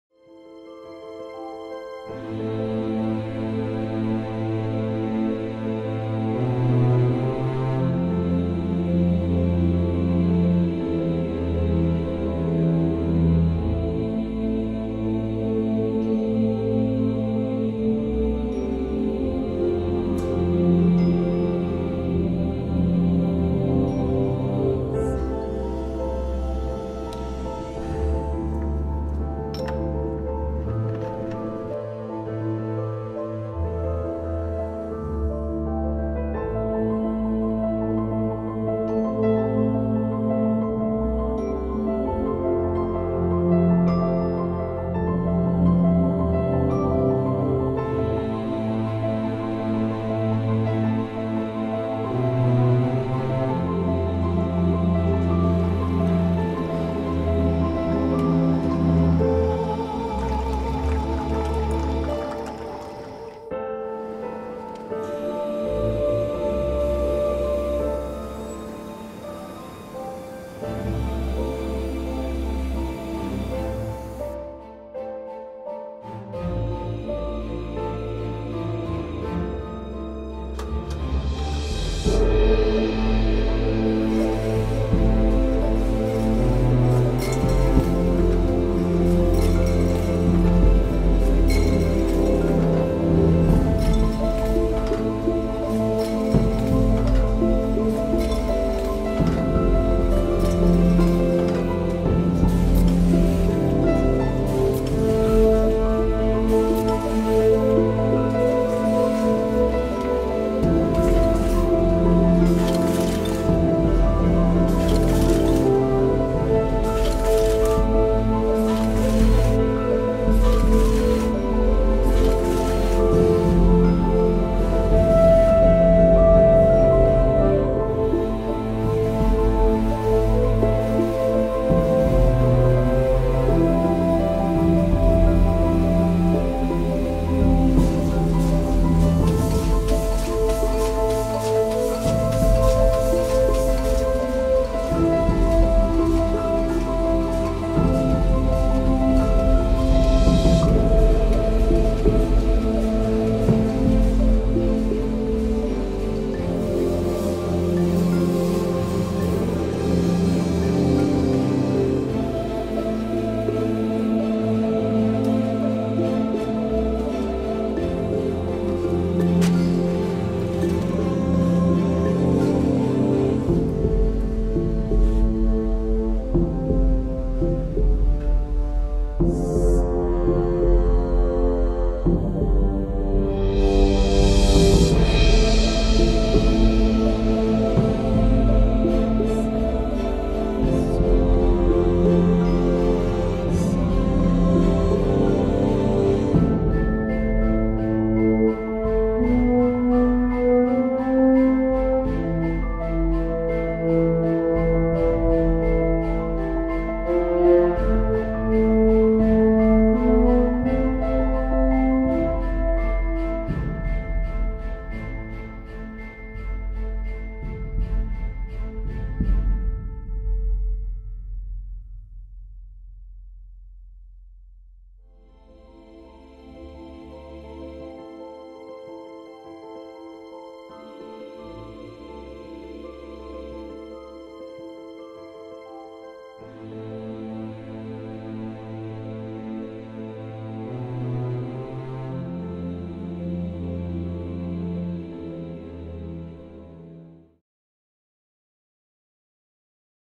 Montana Gold - Birth of a Spraycan
The new Montana Gold colour system has been completely overhauled and optimised. With 204 possibilities, Montana Gold offers the largest and most concise colour range available in spray paint. Montana Gold can be applied on canvas, wood, concrete, metal, glass or even flexible surfaces.

Ever wondered how a Montana Spraycan gets produced? This video gives you a inside look of the production, from the first to the last step!